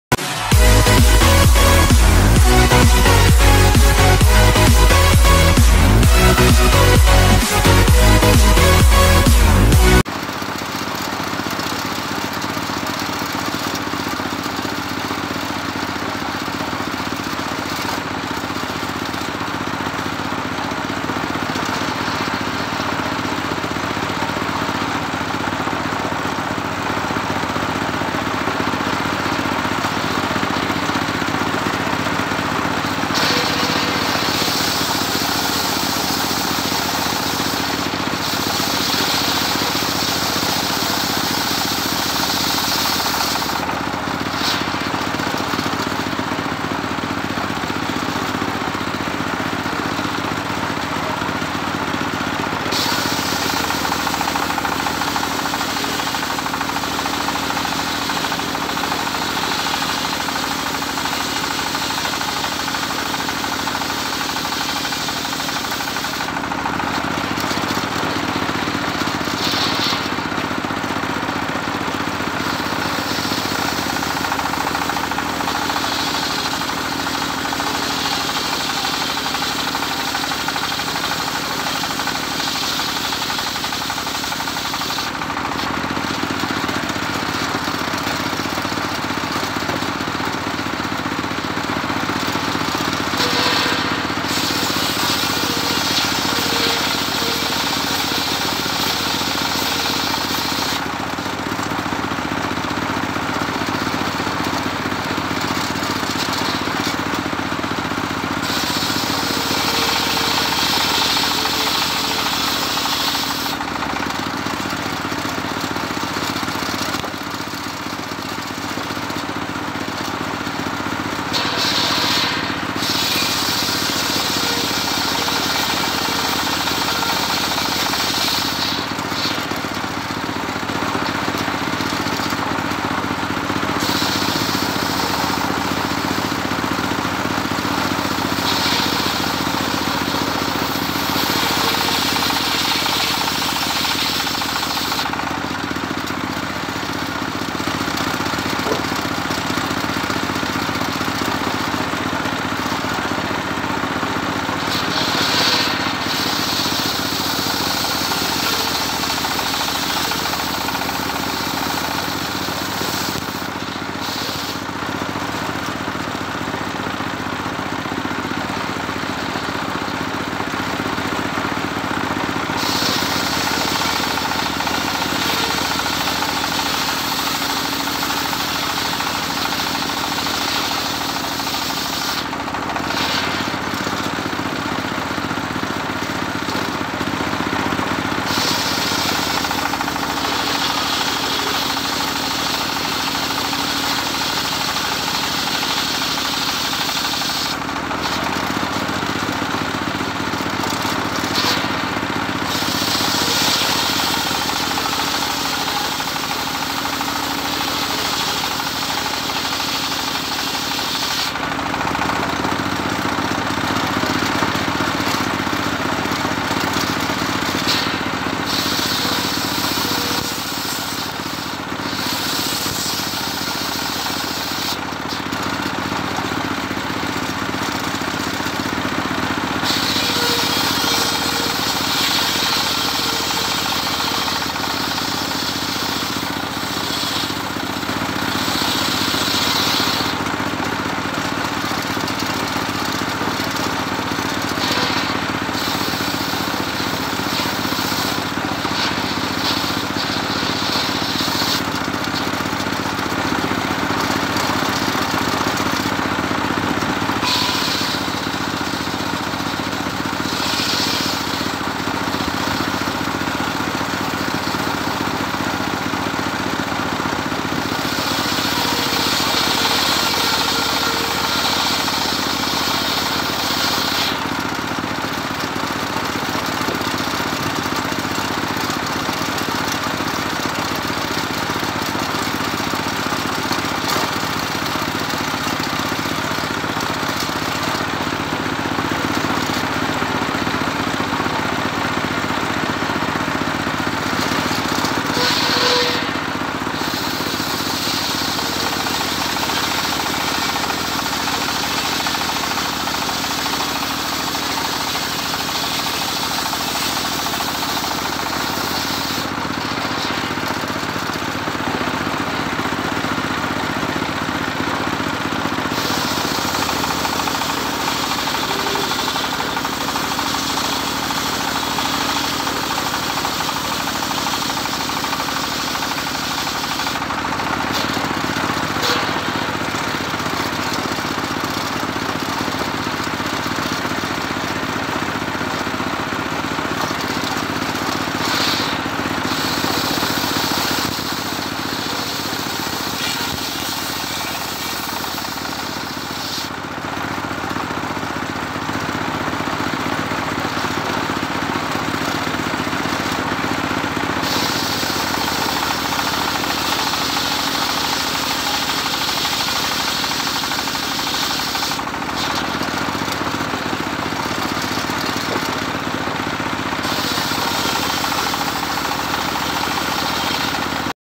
BUTUH TENAGA PRIMA, KAYU MEDANG DI GERGAJI MESIN SERKEL RAKITAN#kayumedang
butuh tenaga prima, kayu Medang digergaji mesin serkel rakitan
proses penggergajian kayu Medang di bikin balok 7x13